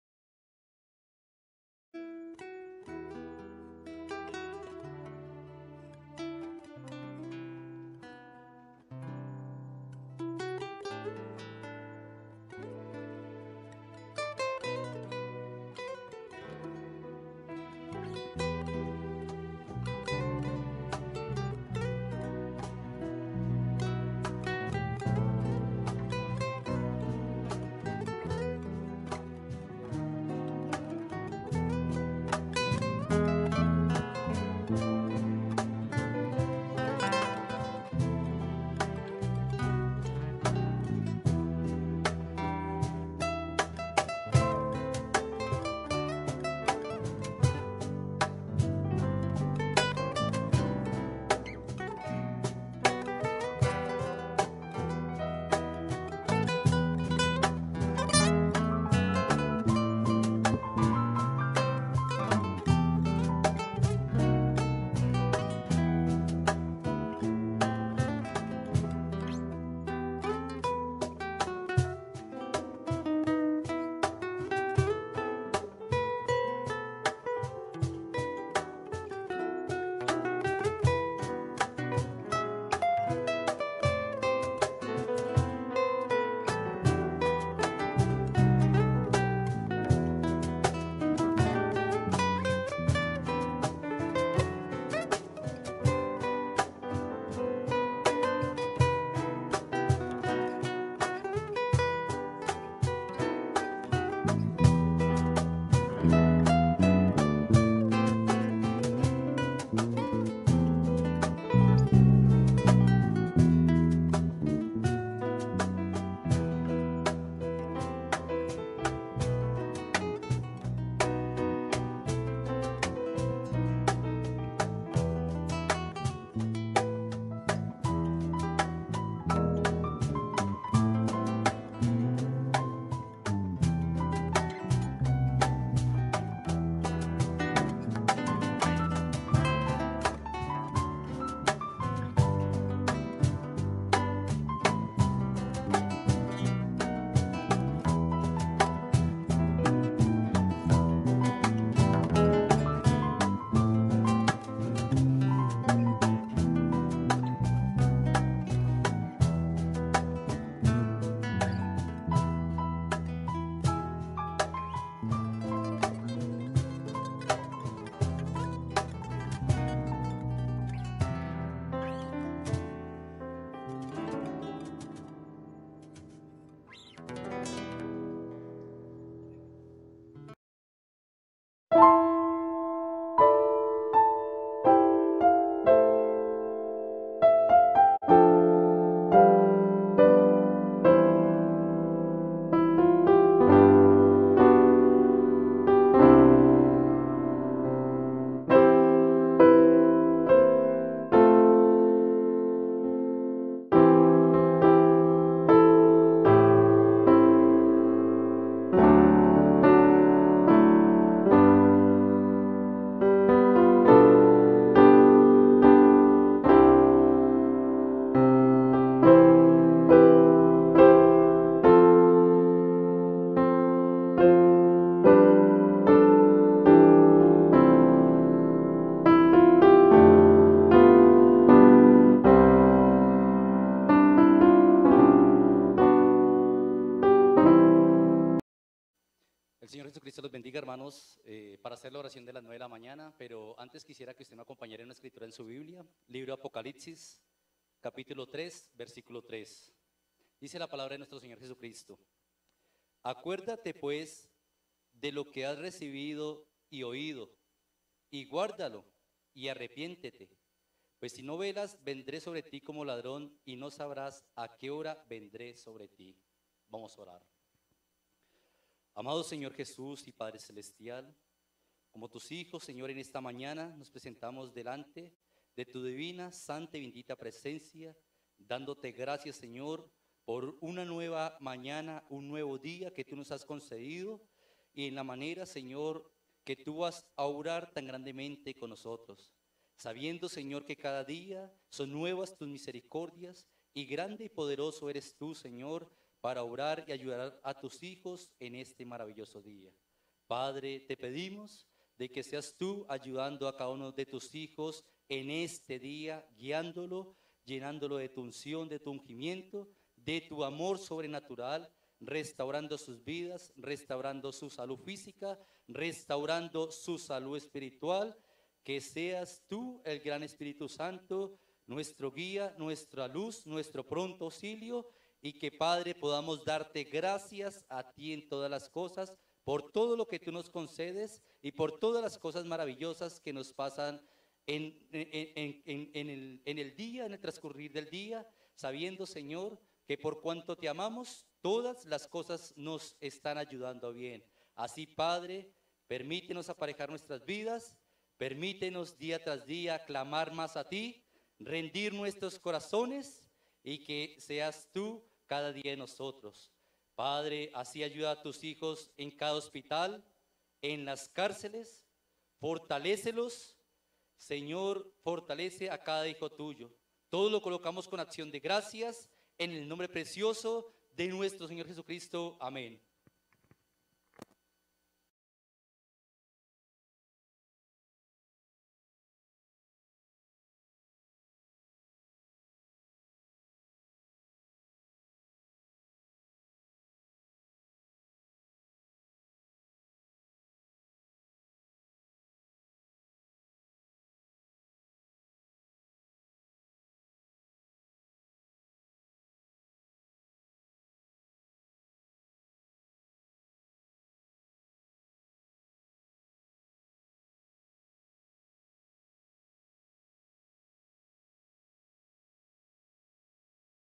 ORACIÓN DE LAS 9AM
JUAN 3:16-17
16 Porque de tal manera amó Dios al mundo, que ha dado a su Hijo unigénito, para que todo aquel que en él cree, no se pierda, mas tenga vida eterna.
17 Porque no envió Dios a su Hijo al mundo para condenar al mundo, sino para que el mundo sea salvo por él.